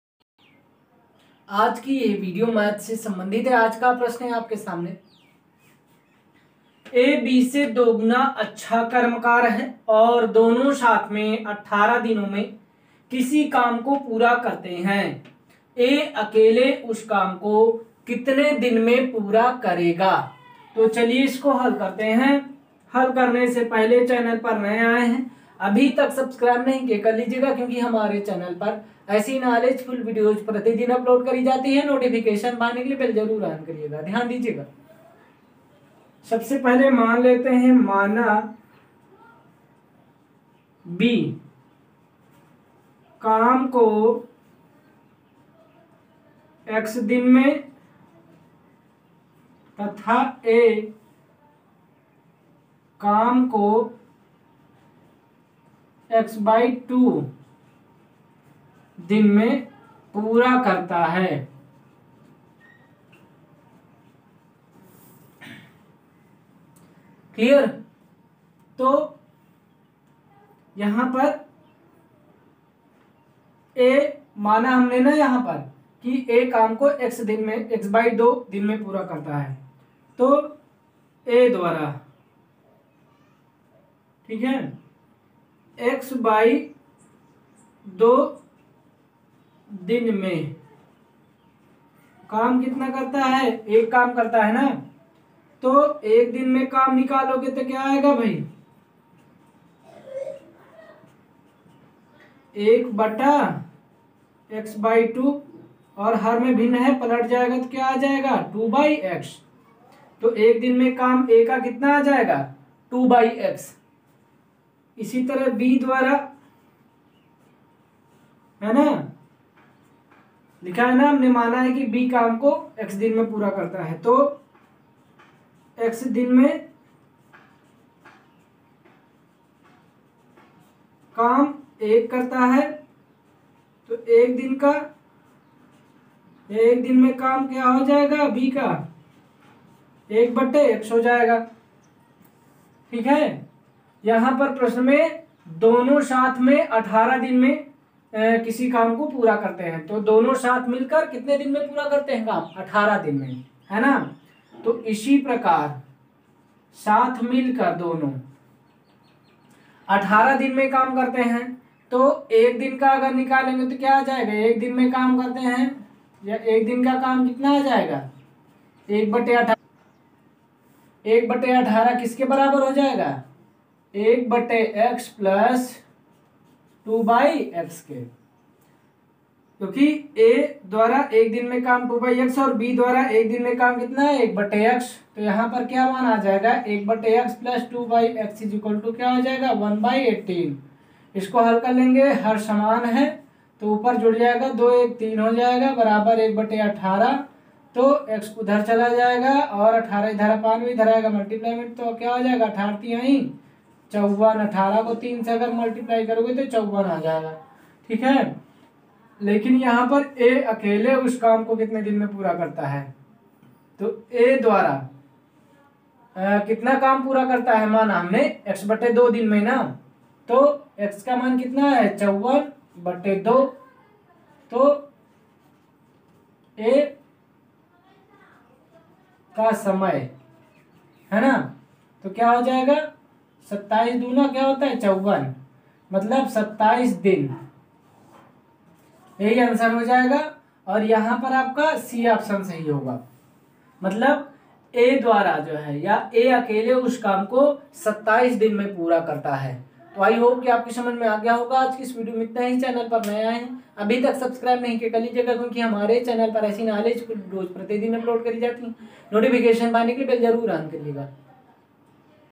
A B से दोगुना अच्छा कर्मकार है और दोनों साथ में 18 दिनों में किसी काम को पूरा करते हैं। A अकेले उस
A B से दोगुना अच्छा कर्मकार है और दोनों साथ में 18 दिनों में किसी काम को पूरा करते हैं। A अकेले उस काम को कितने दिन में पूरा करेगा ?